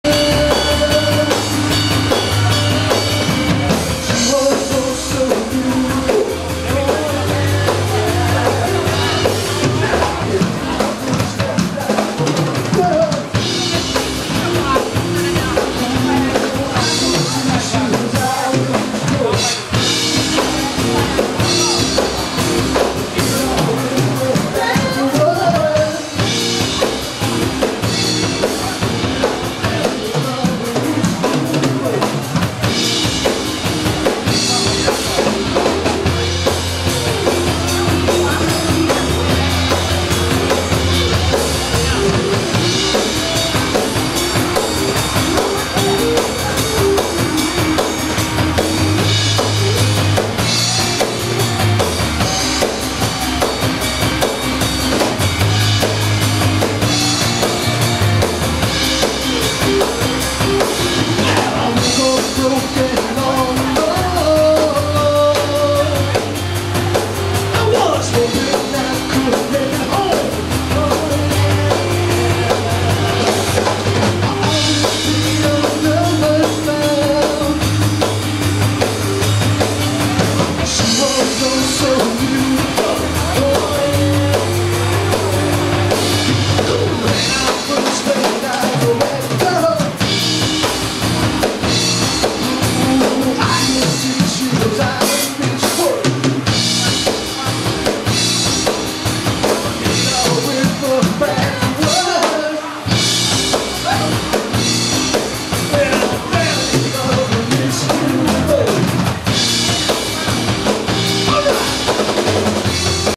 End of Summer Picnic with the Utica Landmark Society, Southwoods, Utica, NY August 2013
Heavy rains that day.